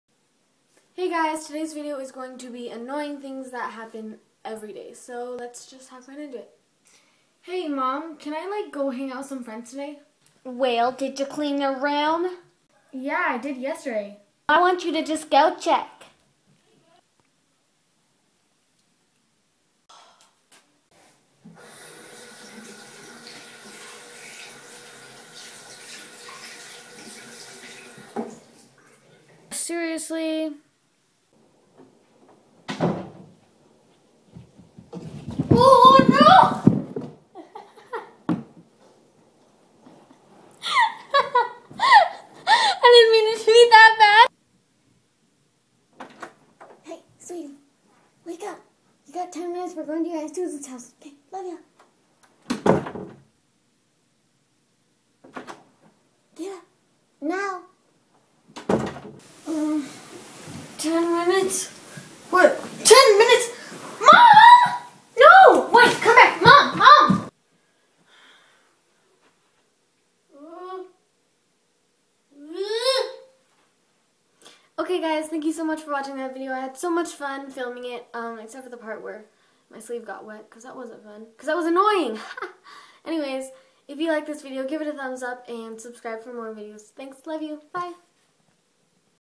Annoying Things That Happen Everyday
My falling all the way down the stairs if my favorite part, what was yours?❤️